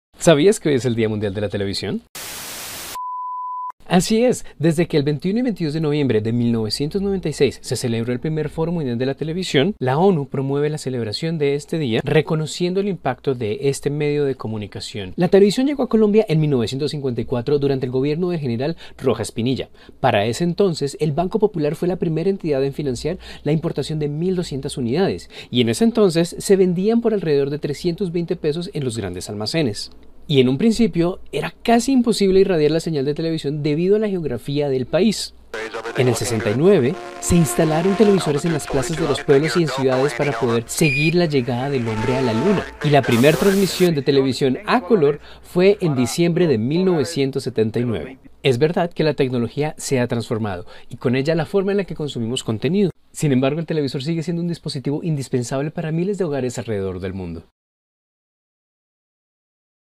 📺🎞️¿Sabías que el 21 de noviembre se conmemora el día mundial de la Televisión? 🤯
Estos son algunos datos sobre la historia de la televisión en Colombia y el mundo.

Cuéntanos cuál era tu programa favorito de tv en tu infancia